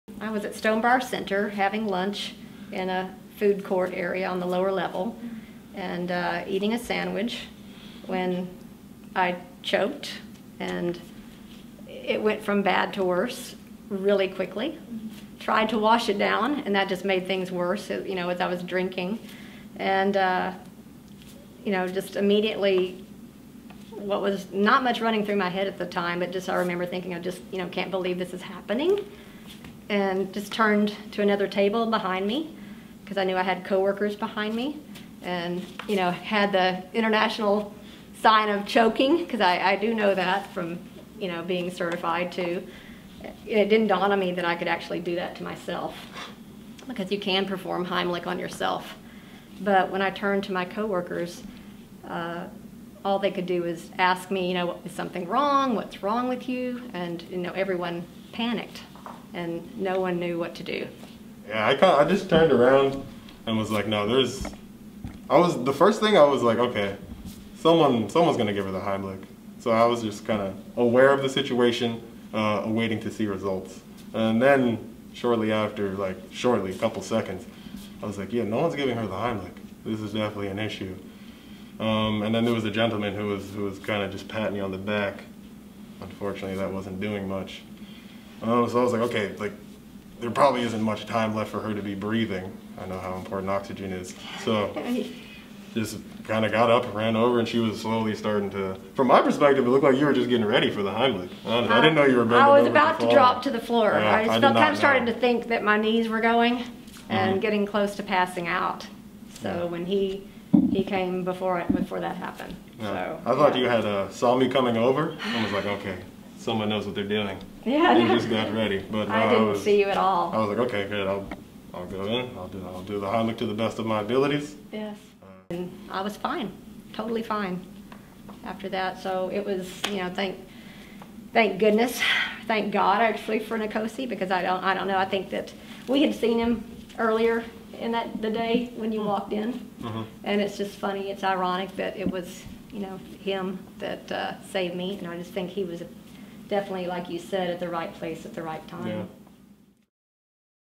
FC Dallas soccer player uses the Heimlich maneuver to save woman's life
Kelly, a retail sales rep, speaks about how FC Dallas soccer player Nkosi Burgess saved her from choking to death by using the Heimlich maneuver at Stonebriar Centre. (DMN video/editing: Tom Fox)